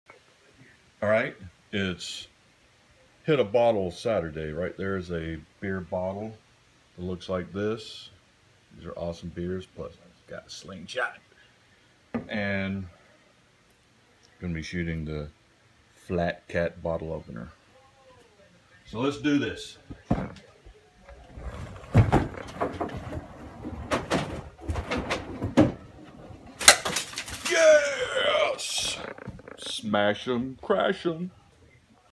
FlatCat Bottle Opener Smashes Bottle
Really Awesome Bottle Opener. But I couldn't resist putting tubes on it to shoot hahaha.